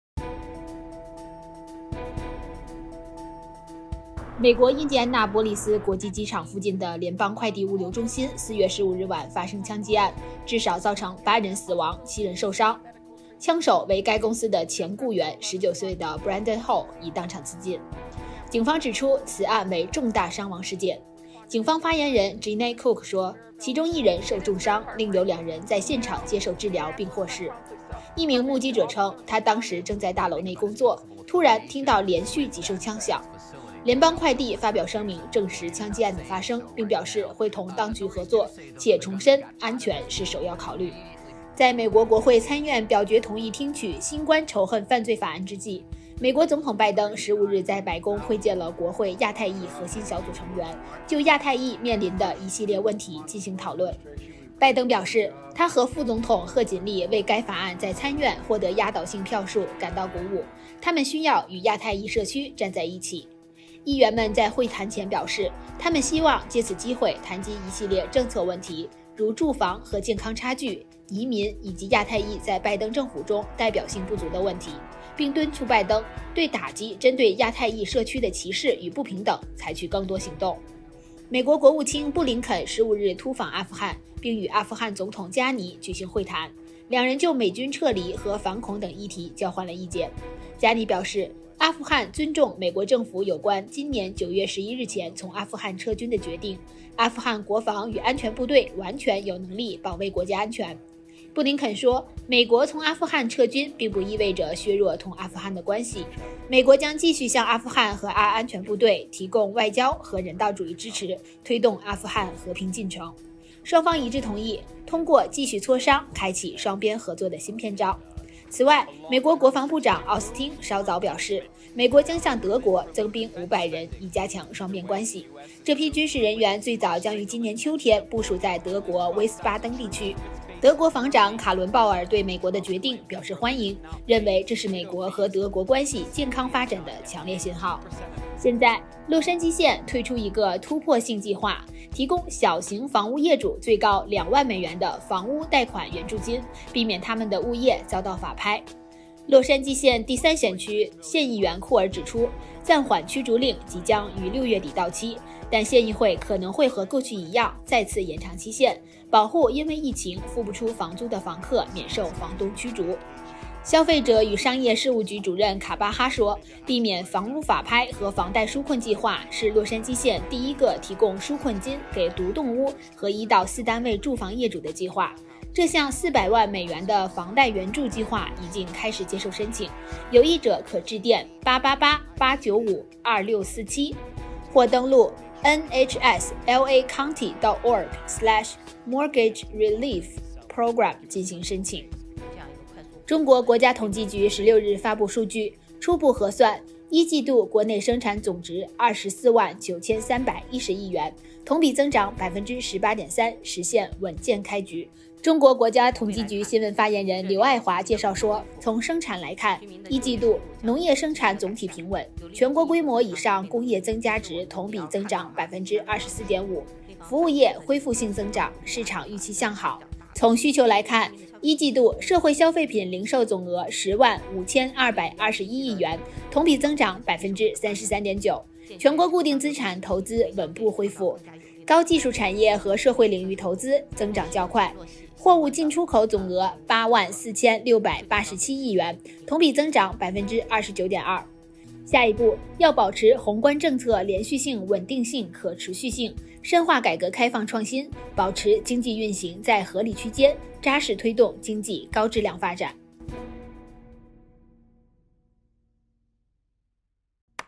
魅力新闻：洛杉矶县推出一个突破性计划，提供最高2万元的房屋贷款援助金
美国印第安纳波利斯国际机场附近的联邦快递(FedEx)物流中心4月15日晚发生枪击案，至少8人死亡，7人受伤。
中国国家统计局16日发布数据，初步核算，一季度国内生产总值249310亿元，同比增长18.3%，实现稳健开局。